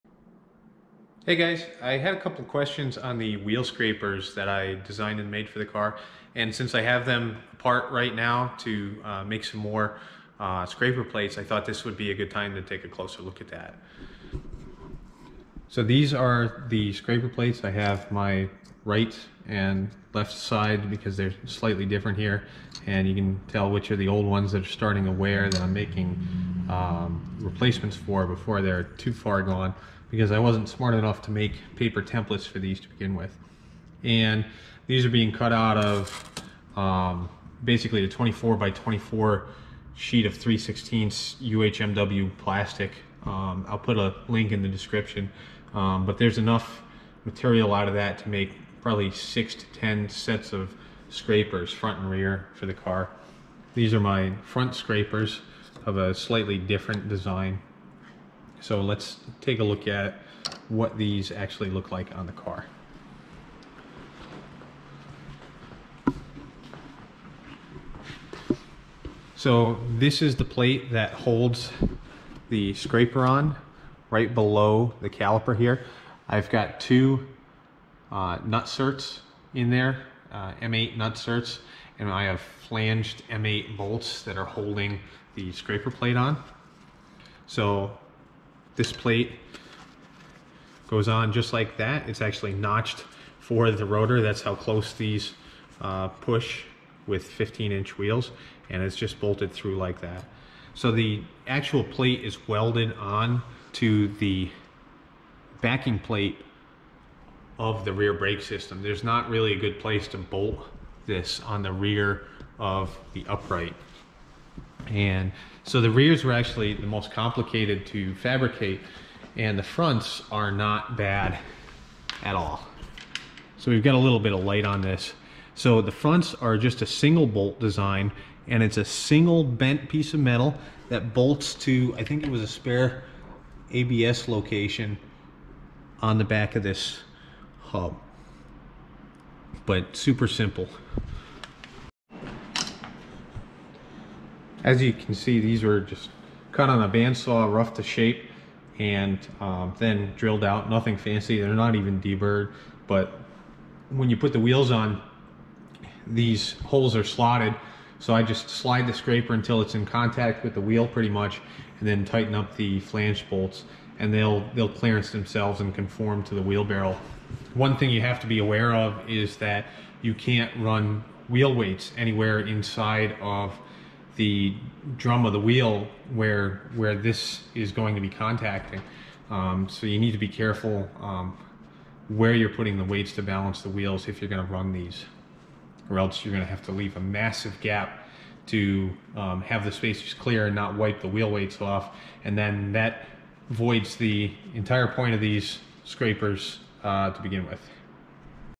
Rally Car Prep- Wheel Scraper Design
Take a closer look at how we've fabricated the wheel scrapers for this 1998 Subaru Impreza to help keep the calipers from being damaged under extreme conditions during rallying.